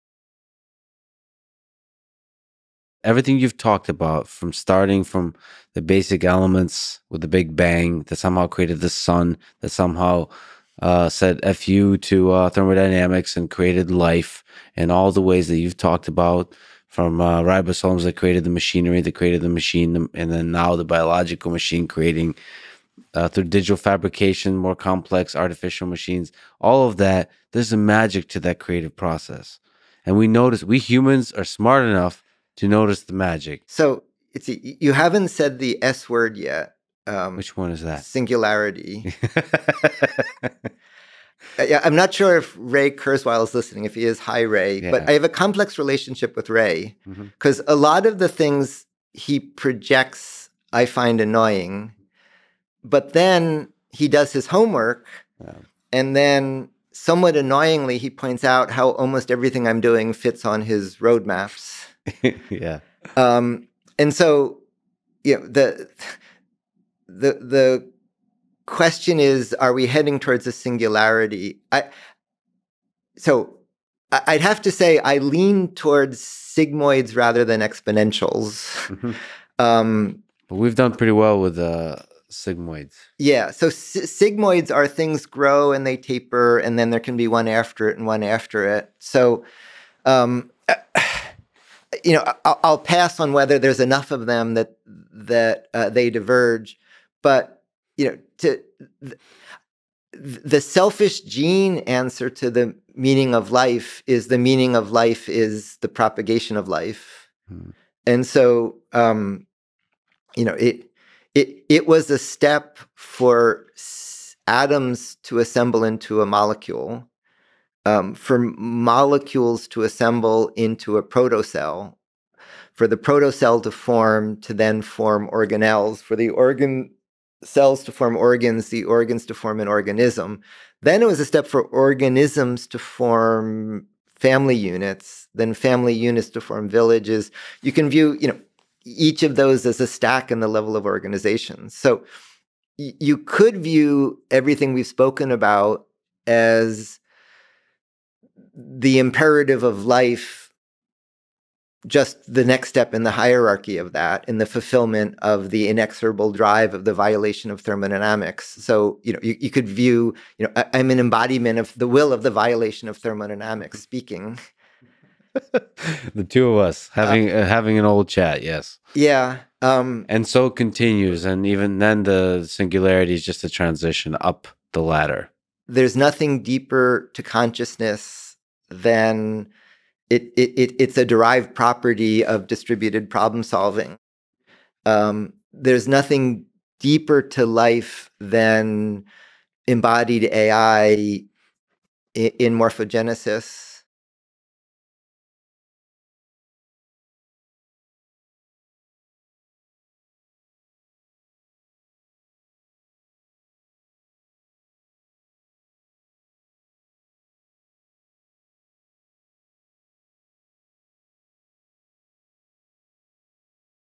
Lex Fridman: Is the singularity coming?
GUEST BIO:
Neil Gershenfeld is the director of the MIT Center for Bits and Atoms.